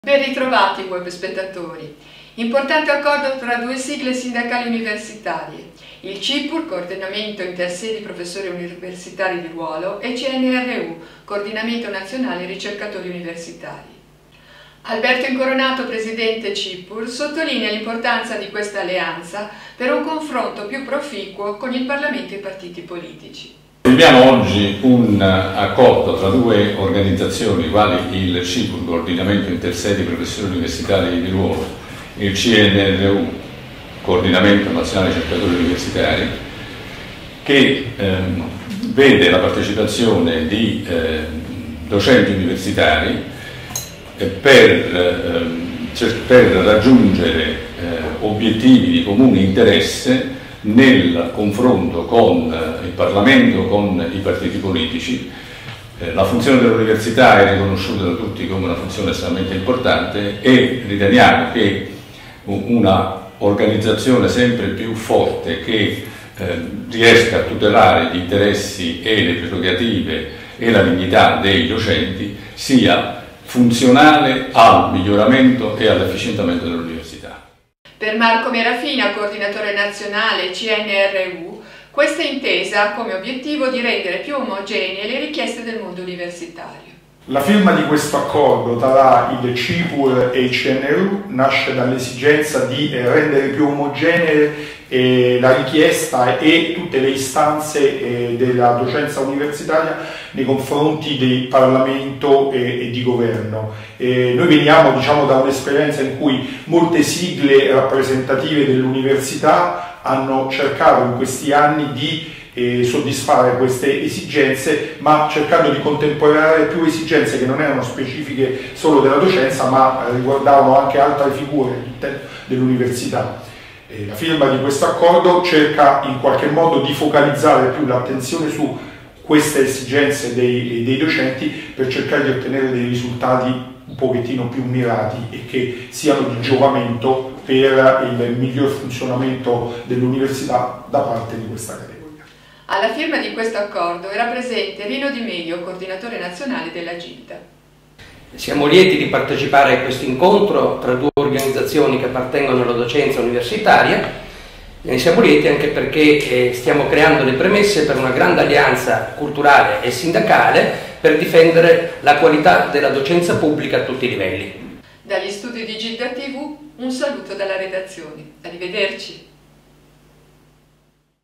UNIVERSITA', SIGLATO ACCORDO TRA CIPUR E CNRU
UNIVERSITA', SIGLATO ACCORDO TRA DOCENTI CIPUR E RICERCATORI CINRU-
Crescono e si rafforzano le alleanze nel mondo universitario. Questa mattina il CIPUR (Coordinamento Intersedi Professori Universitari di Ruolo) e il CNRU (Coordinamento Nazionale Ricercatori Universitari) hanno siglato un accordo che si prefigge l'obiettivo di elaborare percorsi e strategie comuni per valorizzare la dignità e la professionalità dei docenti universitari e migliorare il sistema di istruzione e formazione accademica ...